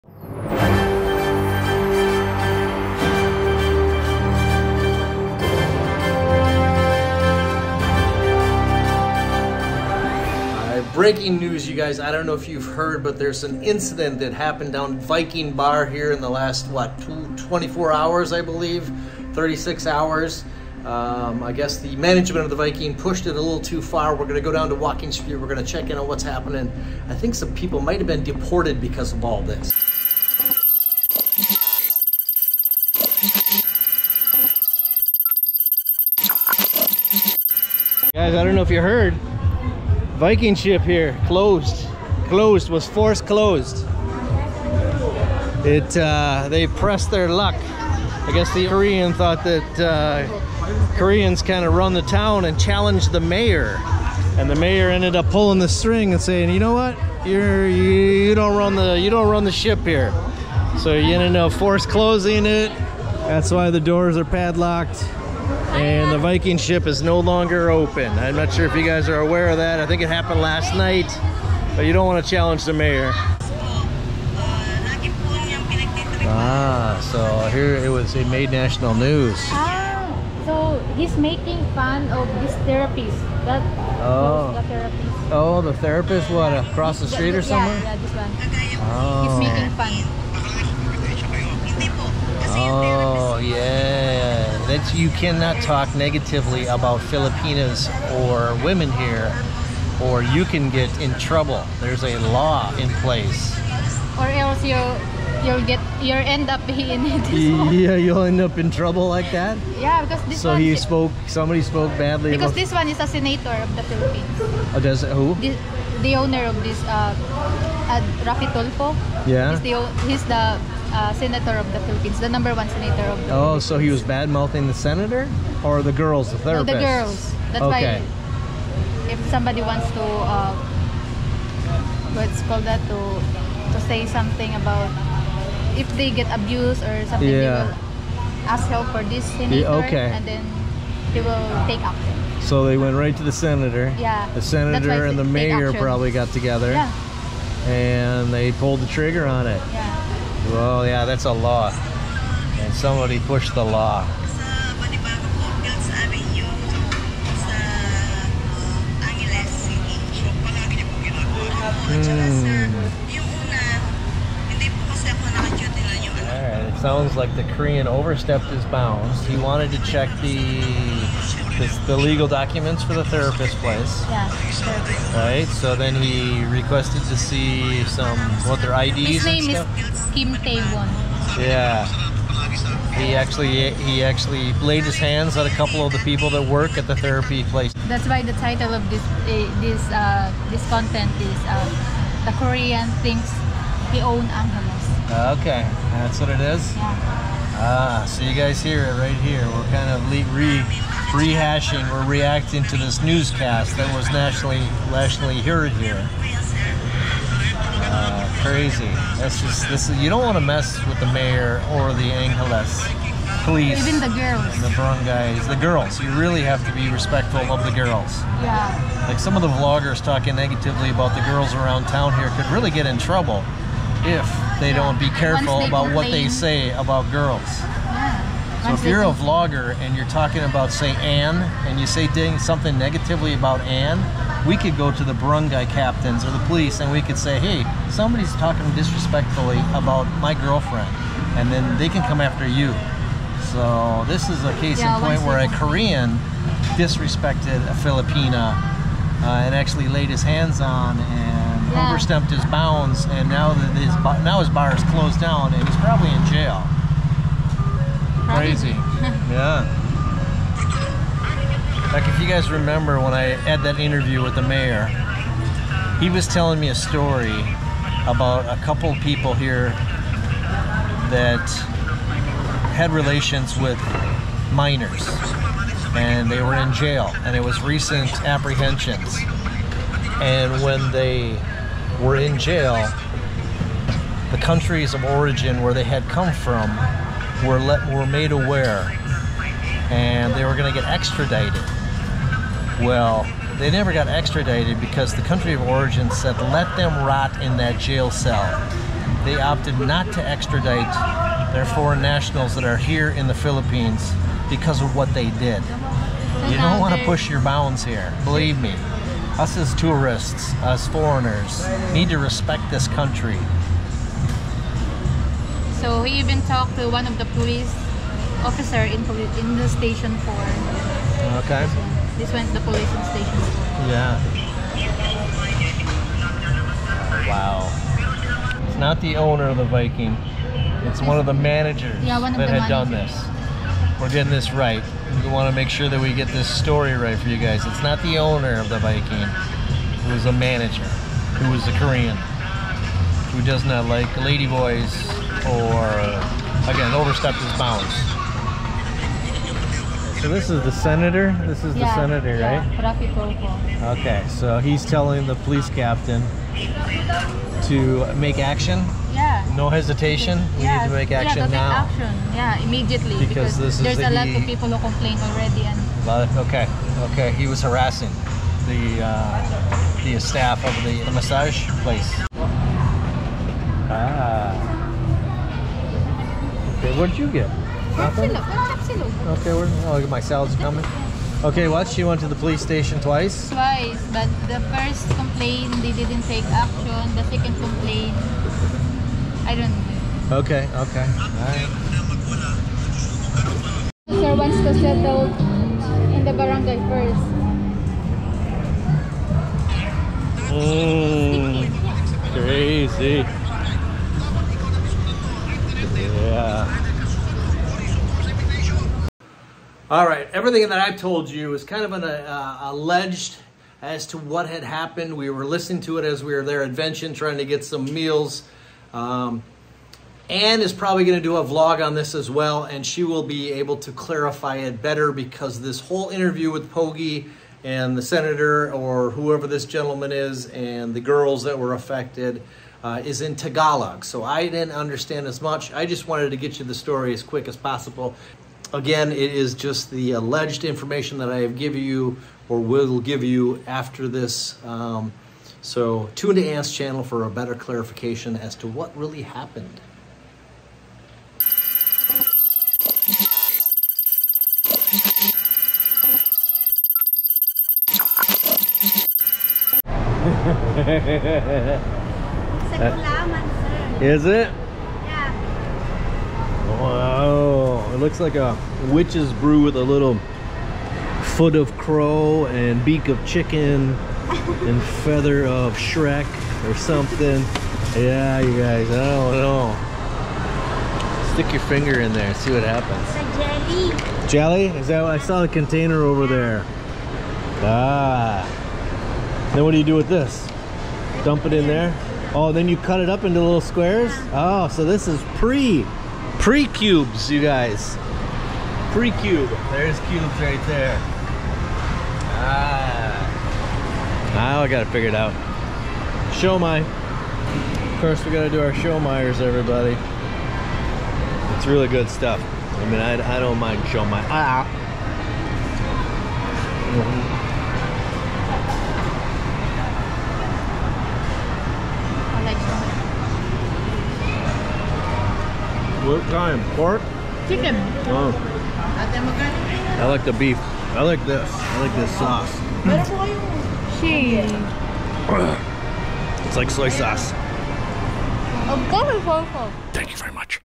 VIKING BAR CLOSED... Korean thought he runs Angeles City but the Mayor said otherwise...
Within the last couple days the Viking Bar on Walking Street was closed because the Korean Manager thought he was the King of the Angeles City... he disrespected some local Filipinos and was put in his place by the local Authorities... this was a huge story on a local newscast.